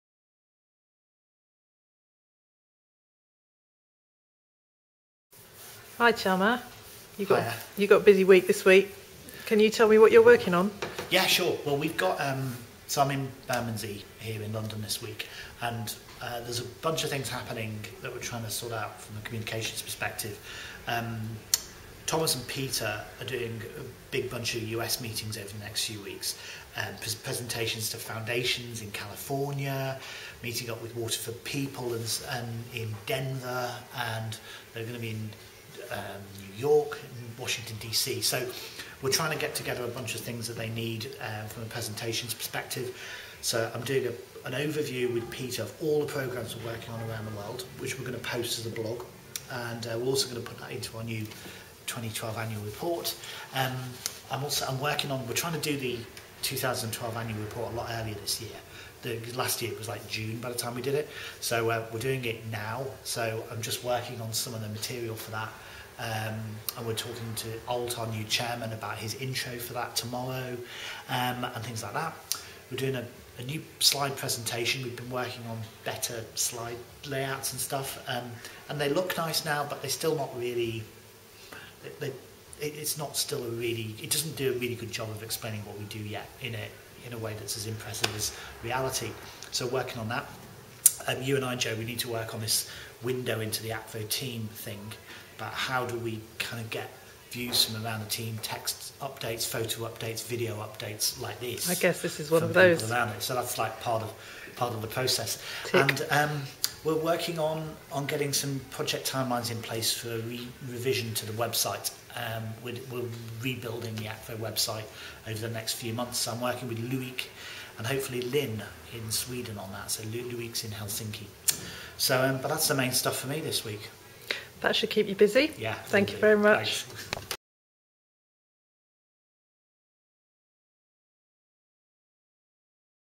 Mark Charmer Akvo update - 19 February 2013
Jo Pratt talks to Akvo communications director Mark Charmer, about what he's working on this week. Bermondsey, London. Tuesday 19 February 2013.